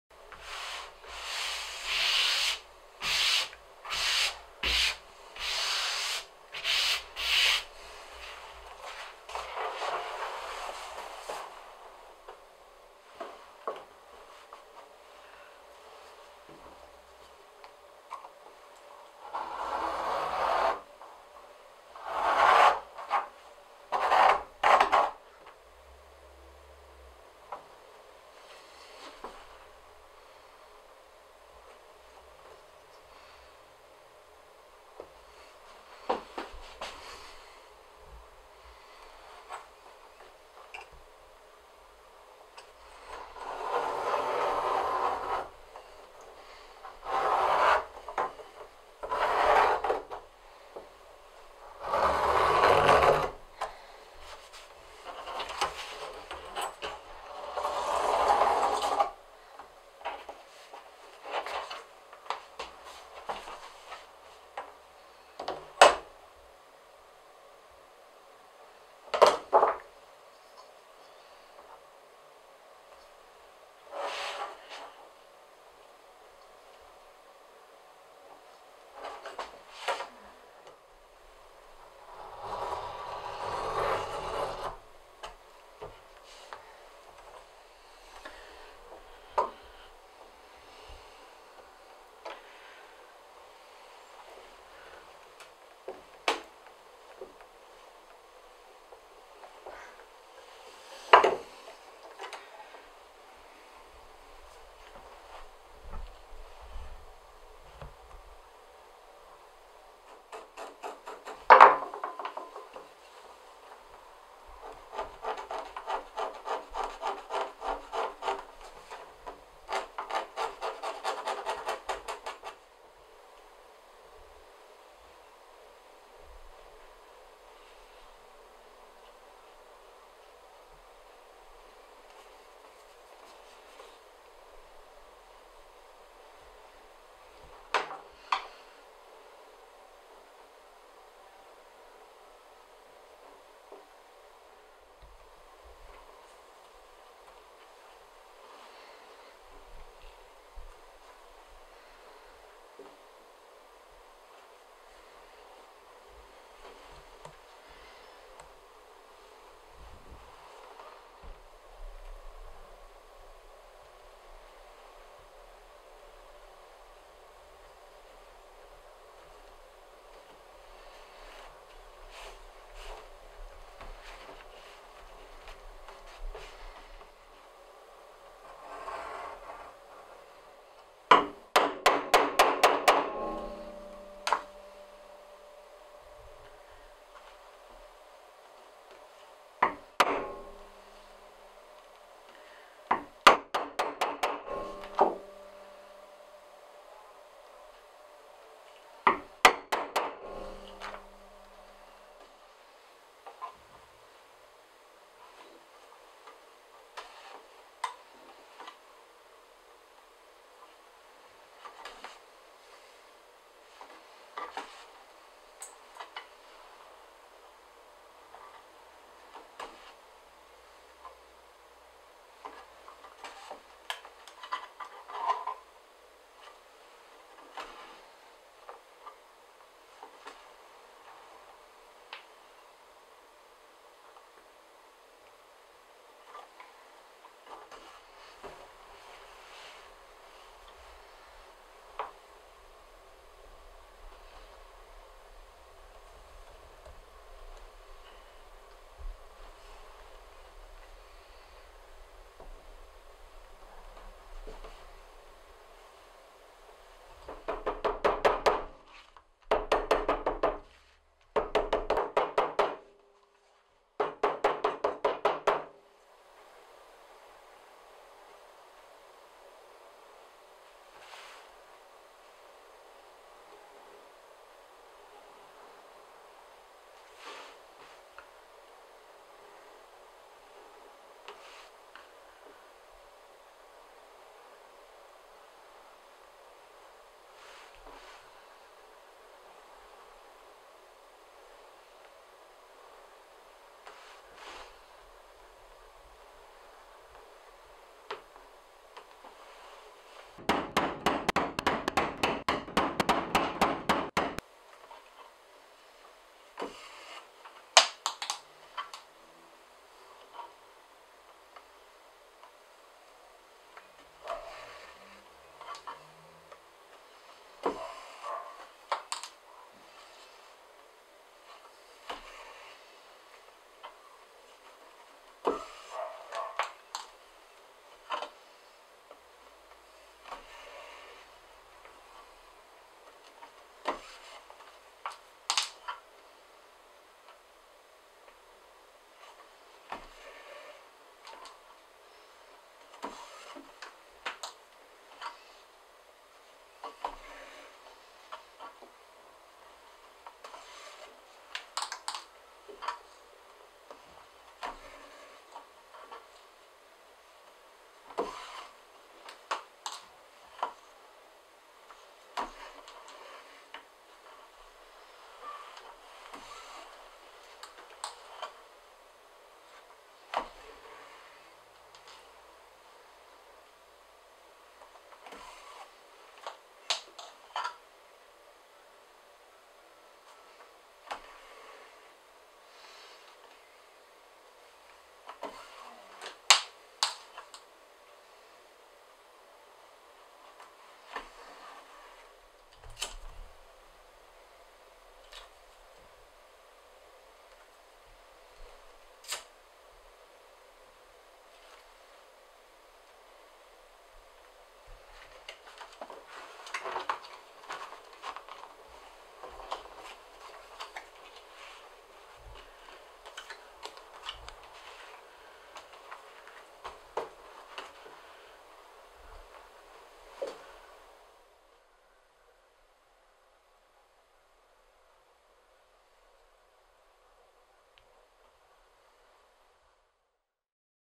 The Making of a Leather Passport Case
Watch the process behind turning finest leather into a watchstrap. Made by hand and using traditional methods, our products are made to last and offer a quality unmatched in today’s world of logos and price tags.

hand​made